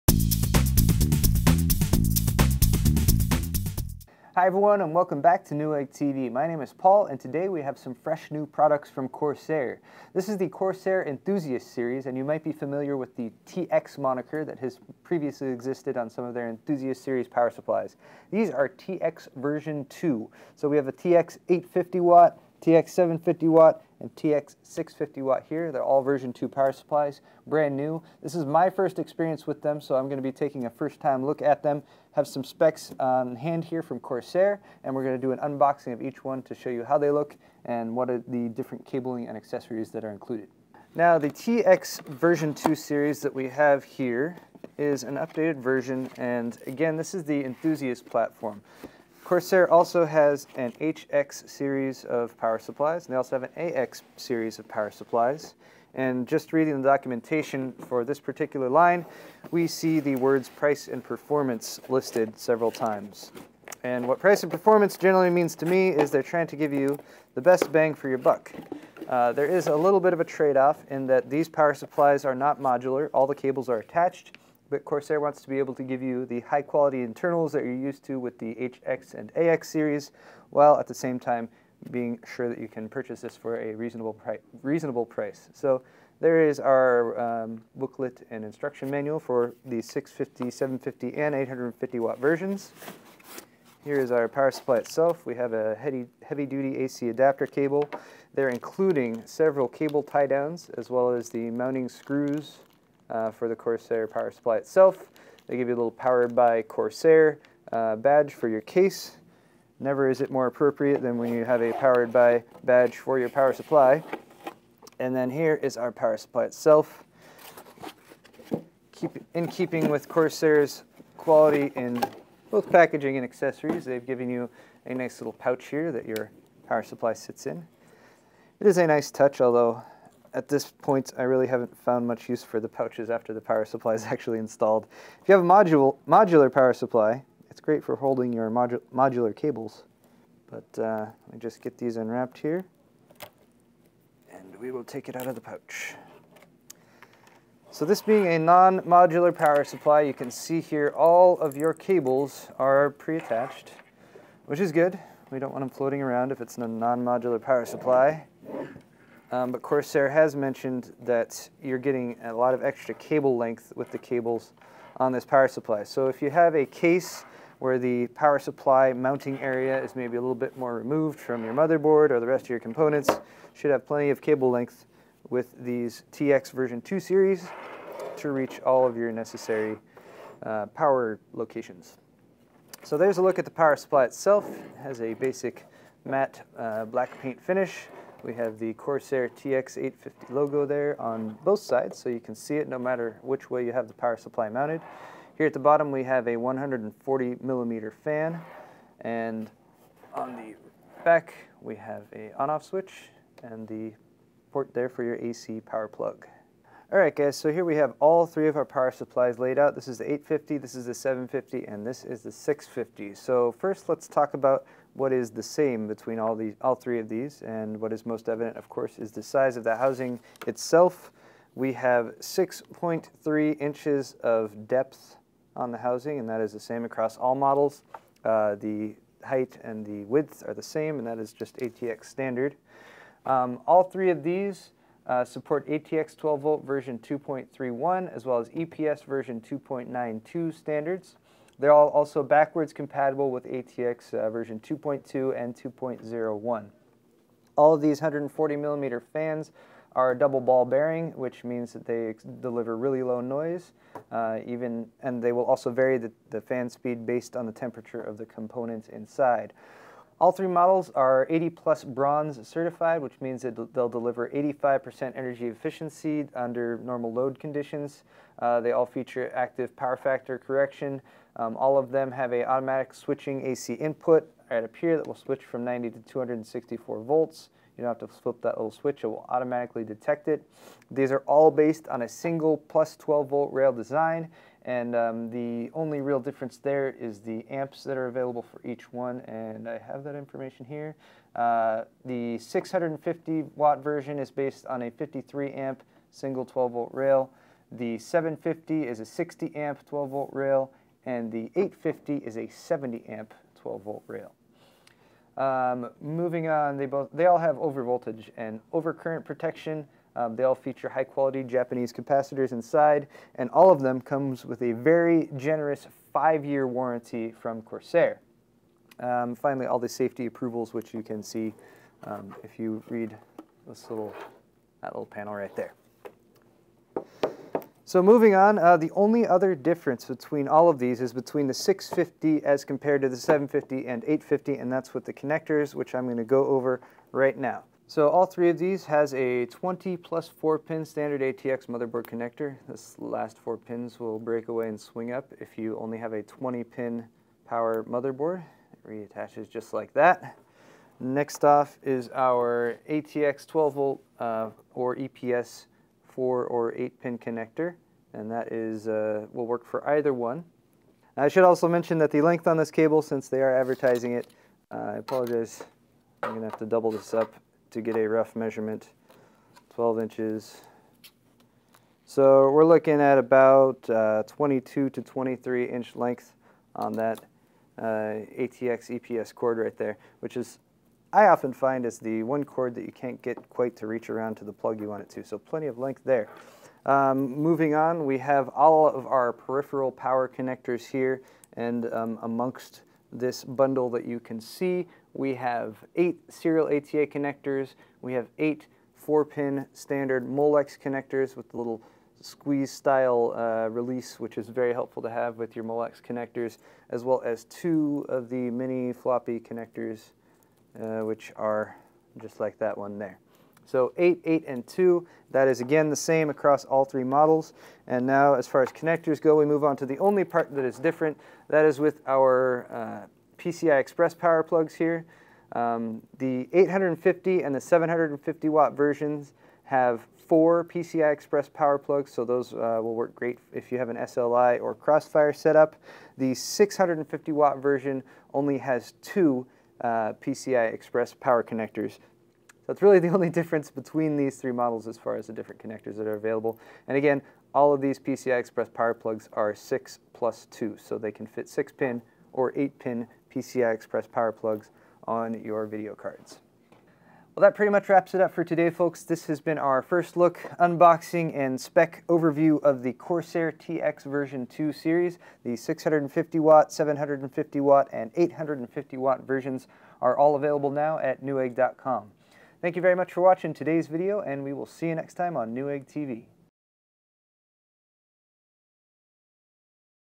Newegg TV: Corsair TX v2 Enthusiast Series Power Supplies - First Look
Corsair is well known for manufacturing high-quality power supplies, and they are continuing this tradition with a 2nd revision of their enthusiast "TX" line (hence the v2 moniker). Available in 650W, 750W, and 850W versions, these PSUs feature a single-rail design, 80 Plus Bronze certification and active power factor correction.

- Credits -
Camera & Editing: Lam
Words, etc.: Paul